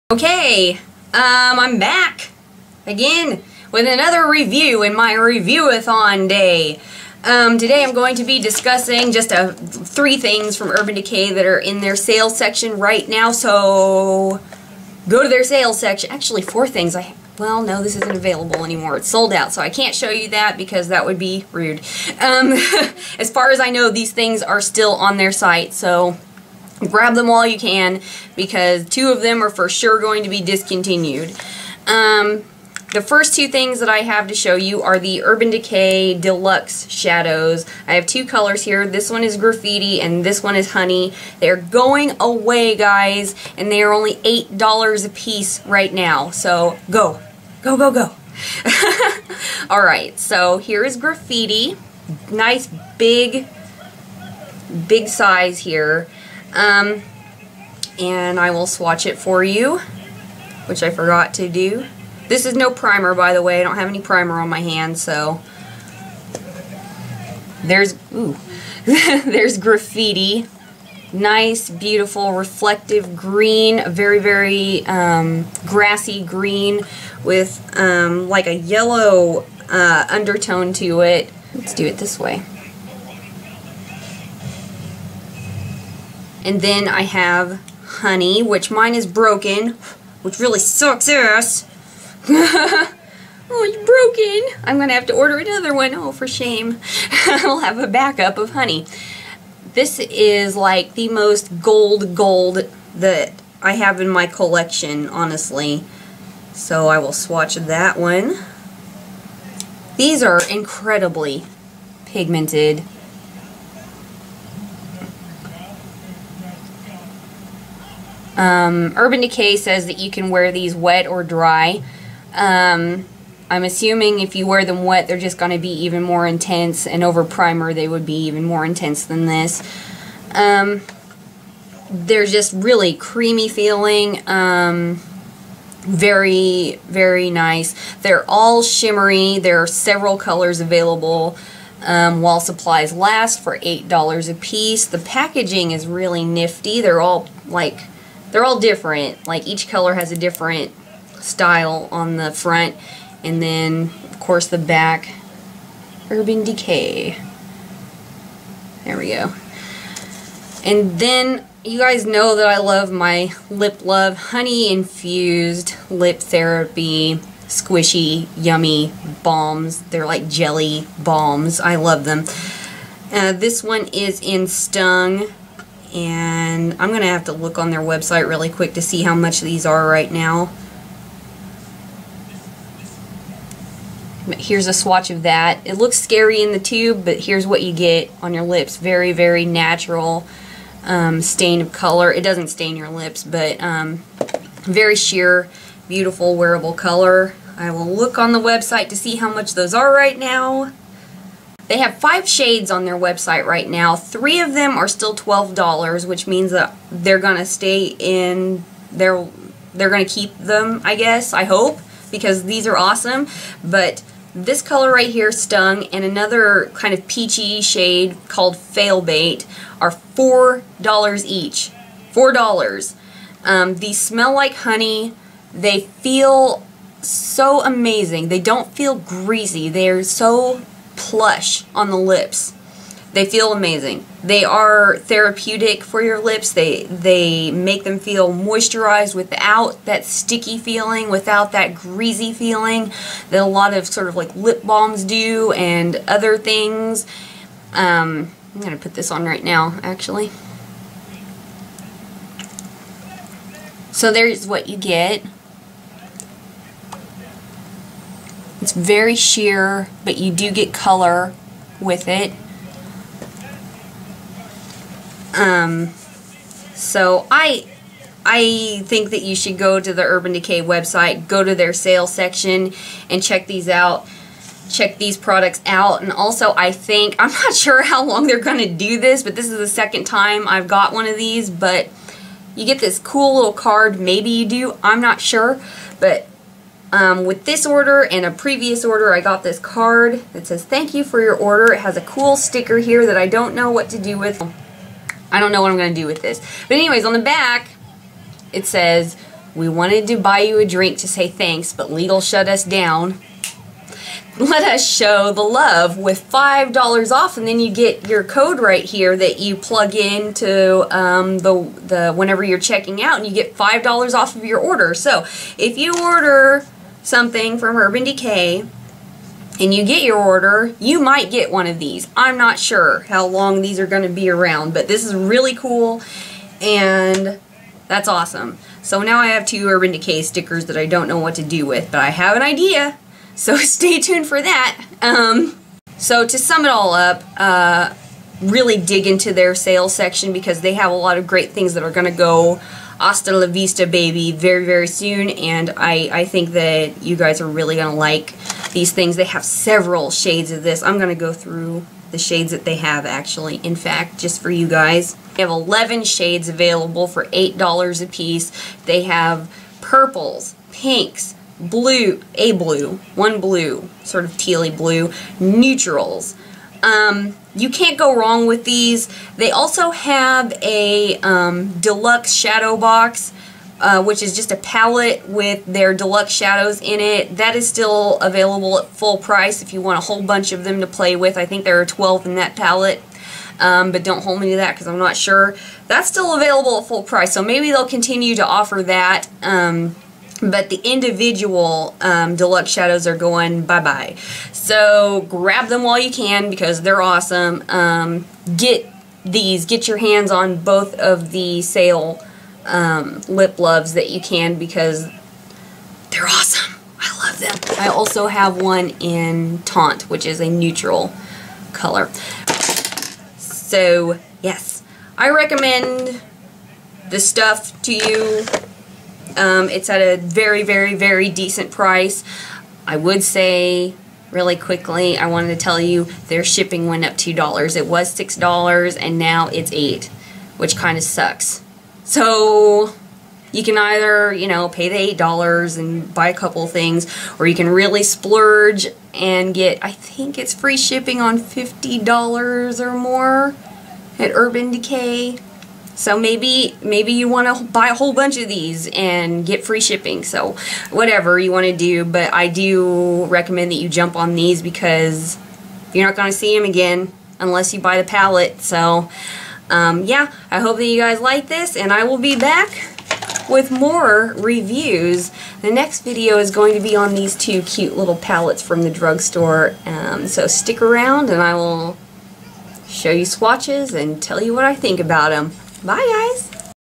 Review-A-Thon! Urban Decay Sale Items (Deluxe Shadows & Lip Love) Swatches & Review
Join me on Twitter or Pinterest...or both!

On my nails:  Takko Lacquer-Jimi's Jacket